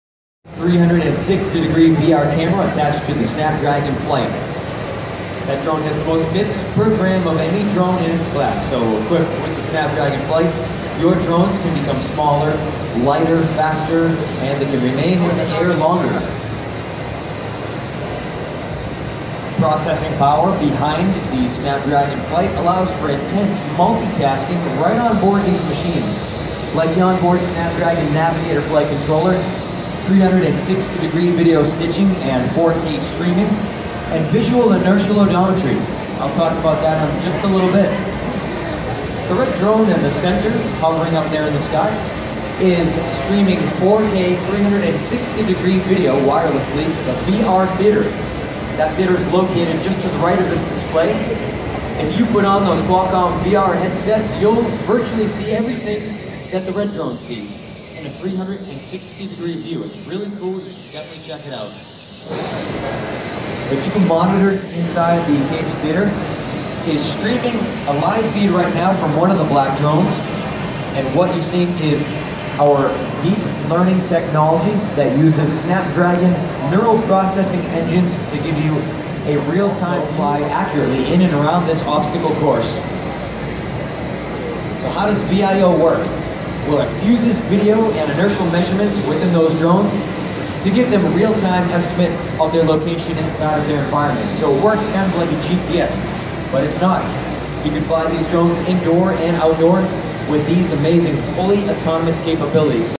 Qualcomm Snapdragon Flight platform CES 2017
Démonstration de la Snapdragon Flight platform présenté par Qualcomm lors du CES 2017